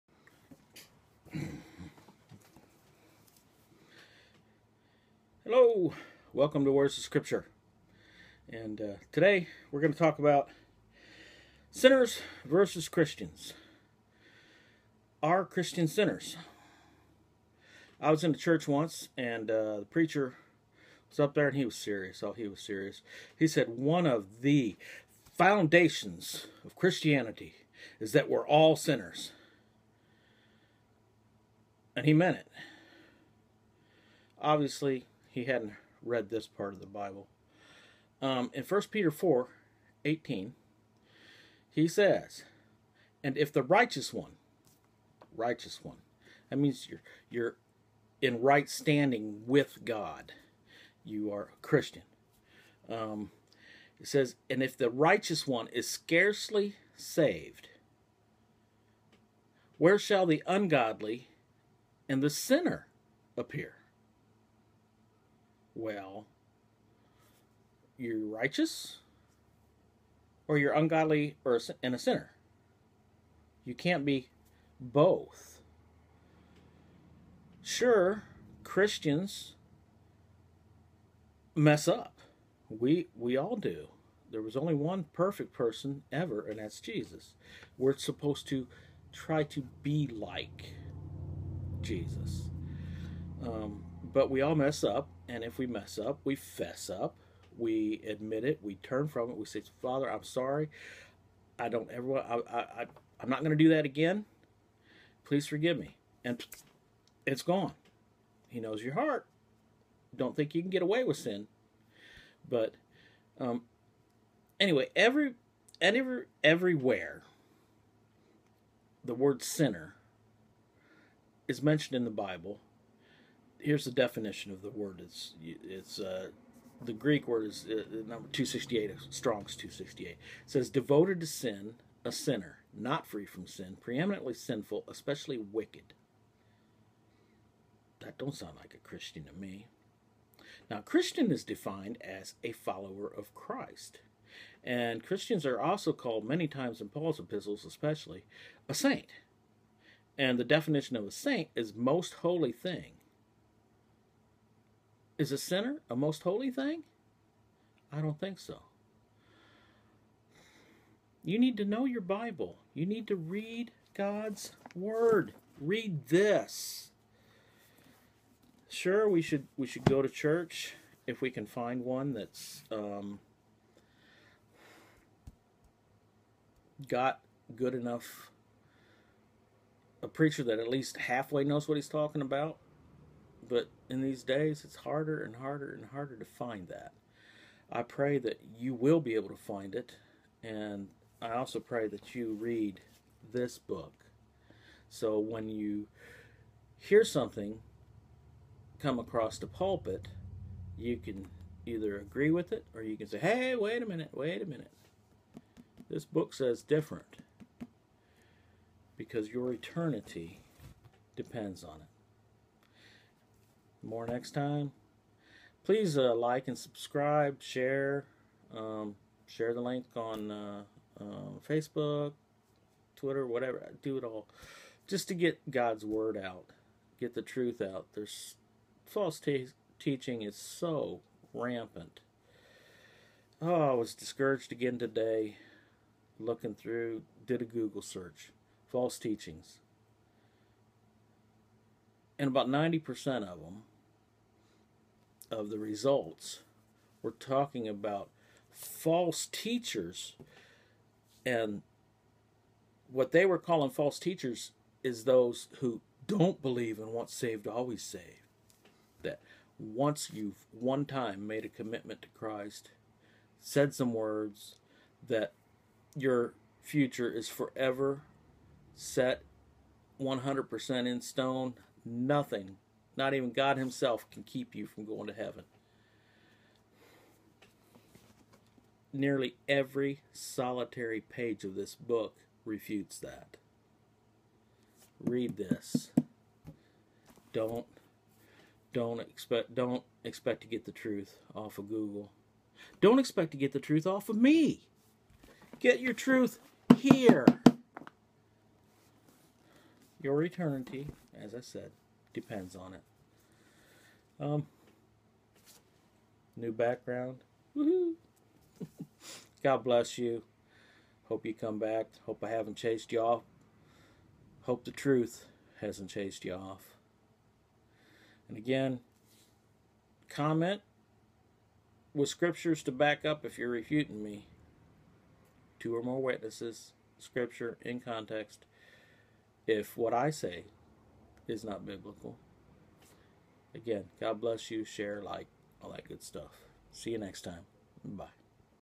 "Sinner or Christian?" Where's the Scripture? 10
Sinner or Christian? Is there a difference? Not according to some preachers and religious people!